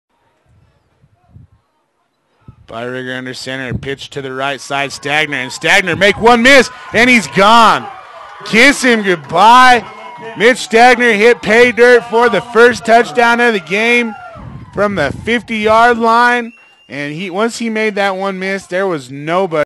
Sangre De Cristo #23 Mitchell Stagner 50yd TD
#23 Mitchell Stagner from Mosca, CO. runs it through the defense for a gain of 50yards making the first touchdown of the game.